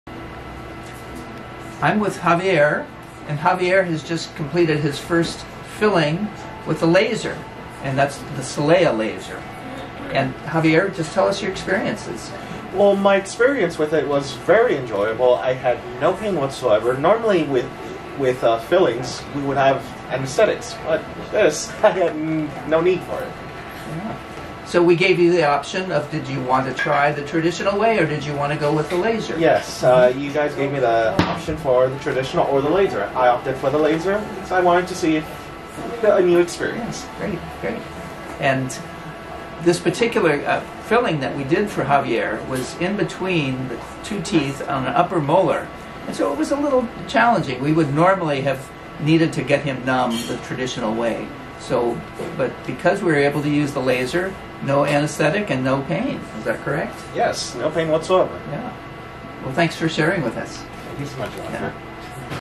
Solea laser! Filling with no shot or drill!
One of our patients just had a cavity treated by using the newest state-of-the-art technology, the Solea laser. It is able to replace the use of the dental drill in many cases!